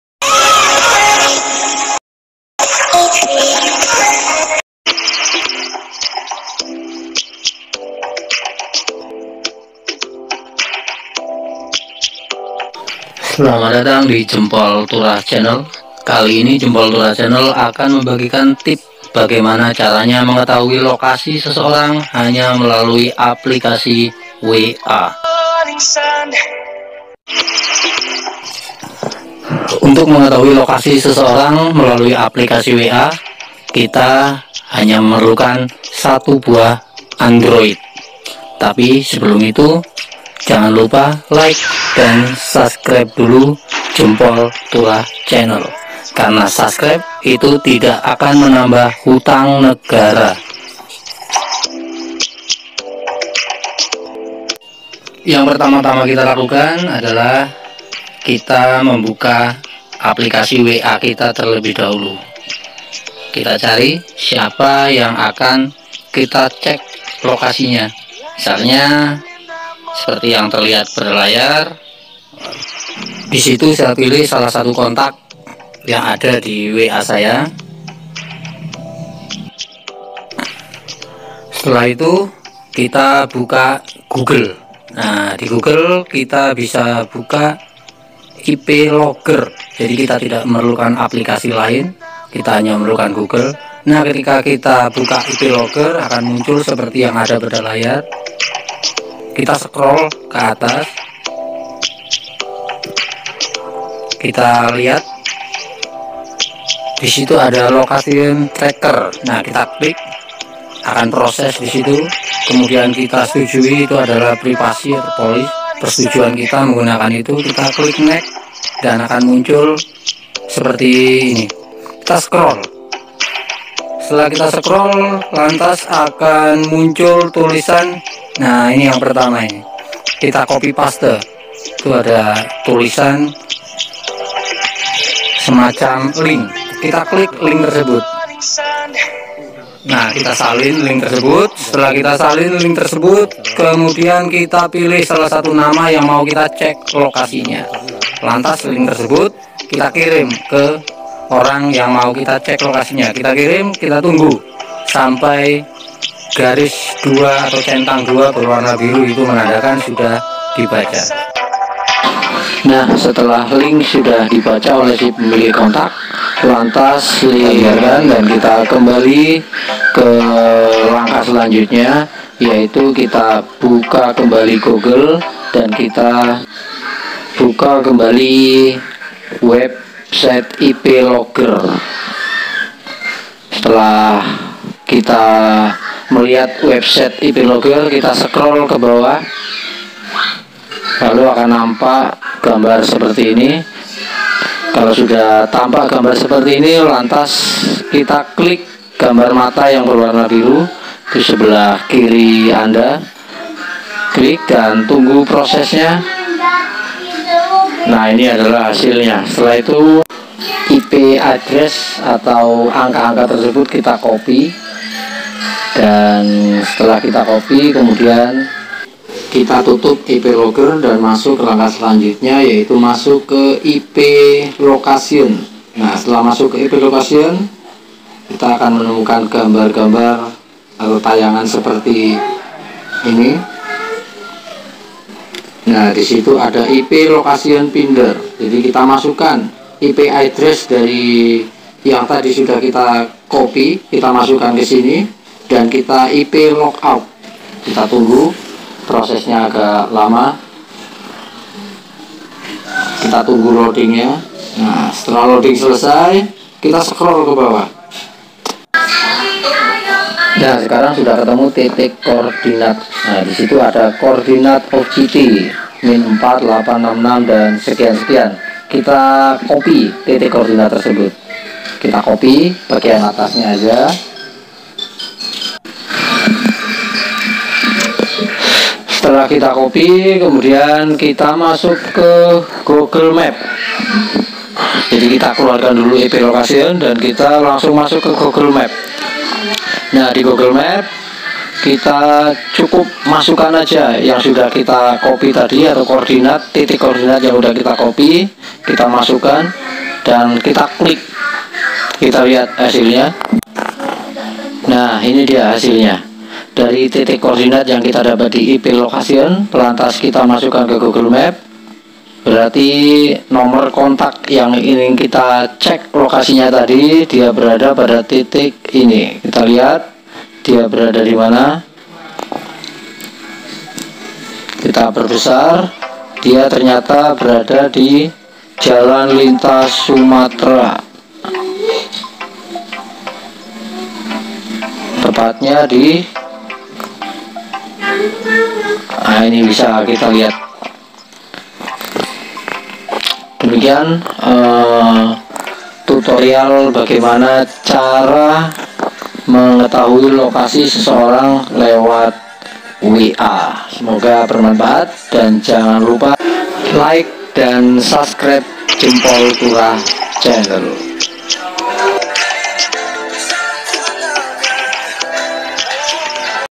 Ini Dia Cara Mengetahui Lokasi Seseorang Hanya Melalui Nomor Kontak Whatsaap (WA) : TUTORIAL
Jempol Turah kali ini akan membagikan Tutorial, bagaimana caranya mencari atau mengetahui lokasi SESEORANG hanya Melalui Kontak WA tanpa perlu mendownload aplikasi macam macam lewat playstore.